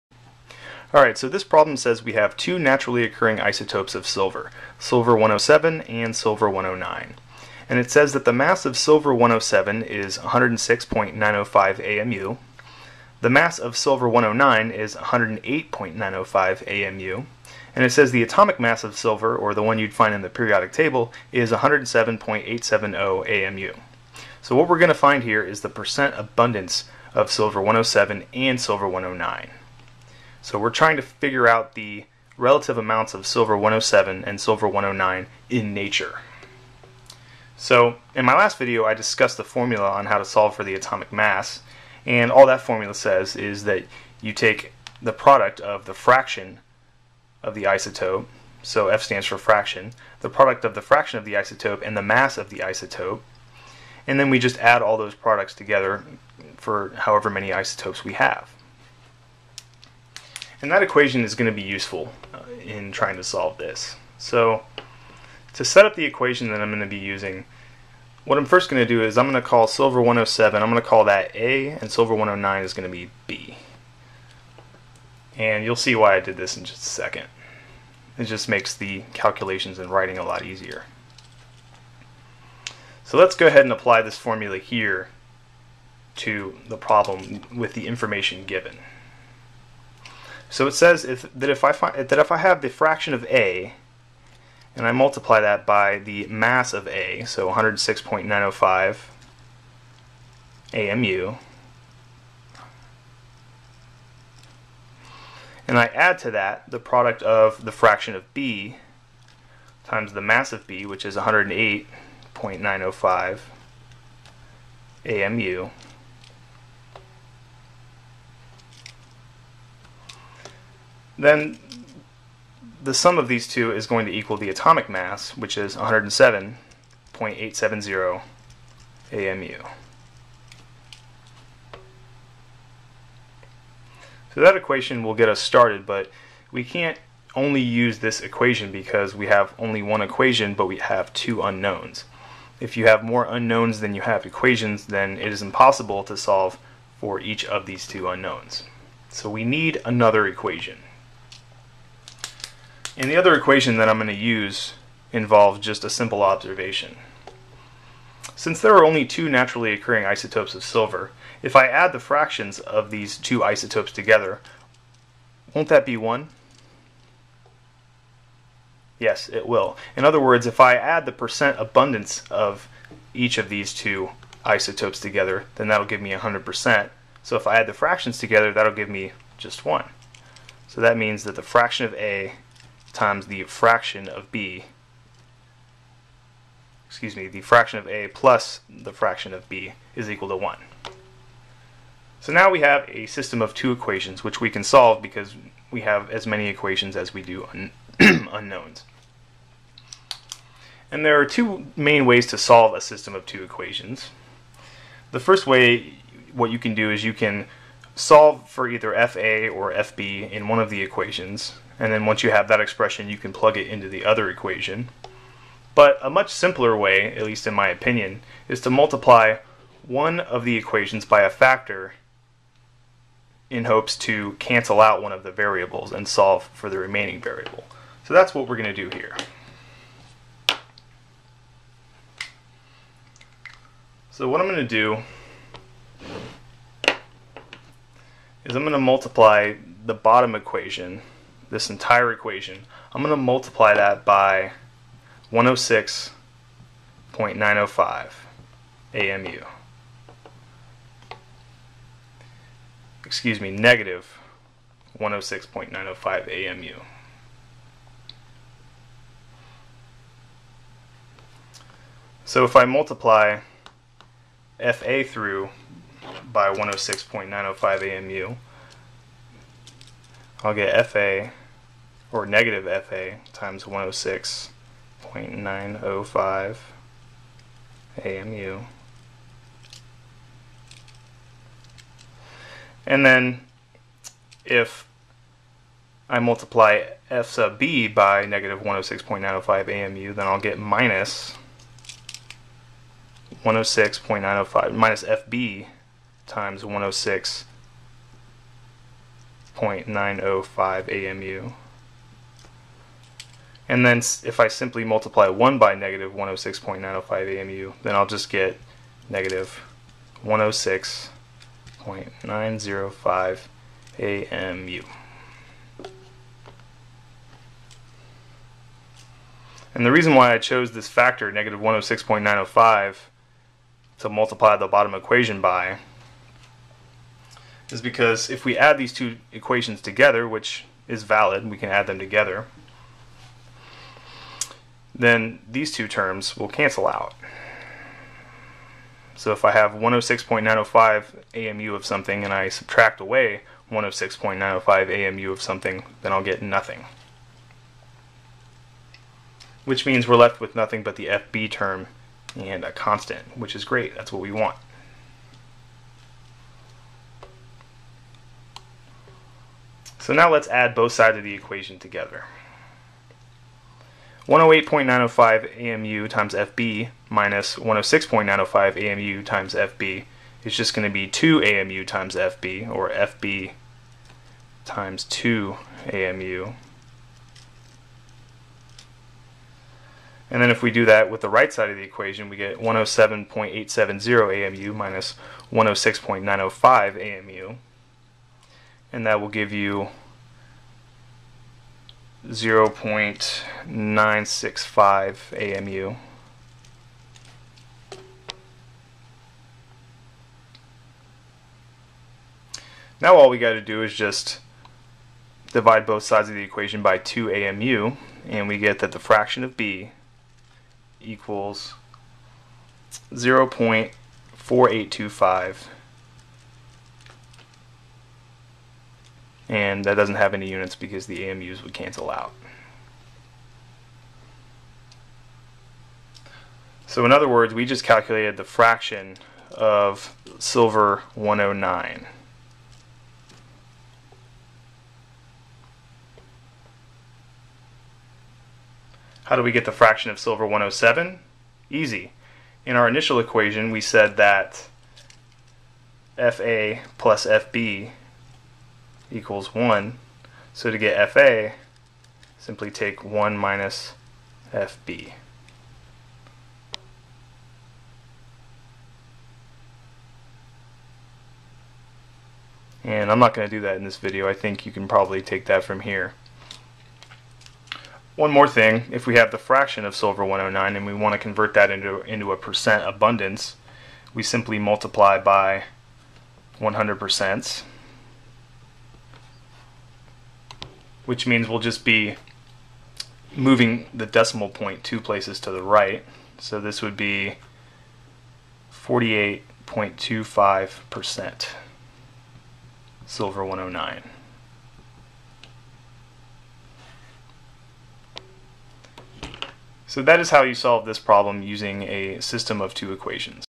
Atomic Mass and Isotopes: Harder Example
If an element has two naturally occurring isotopes, and the atomic mass of the element and the atomic mass of both isotopes are known, then the relative amounts of those two isotopes in nature can be found.  NOTE:  this problem requires you to be able to solve a system of two equations and two unknowns, so brush up on your algebra if you need to.